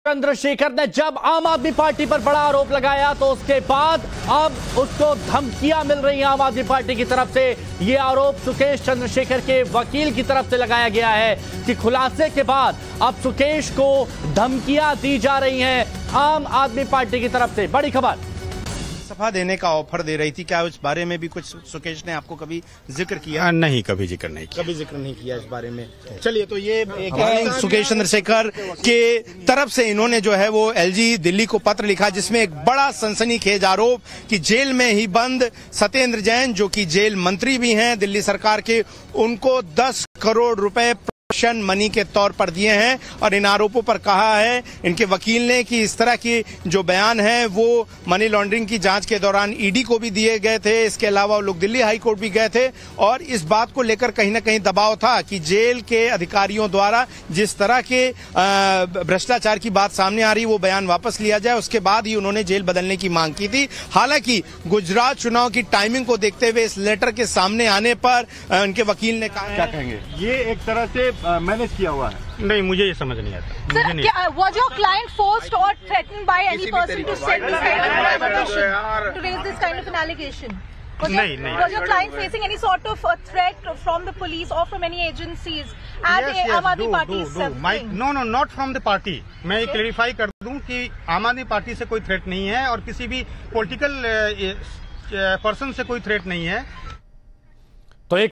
Sukesh Chandrashekhar के वकील का AAP पर बड़ा आरोप- 'खुलासे के बाद सुकेश को मिल रही धमकी'
Sukesh Chandrashekhar के वकील ने Aam Aadmi Party पर बड़ा आरोप लगाया है। उन्होंने कहा, 'खुलासे के बाद सुकेश को धमकी मिल रही है।' बता दें कि सुकेश चंद्रशेखर ने Satyendar Jain पर 'प्रोटेक्शन मनी के नाम पर 10 करोड़ की महाठगी का आरोप लगाया है'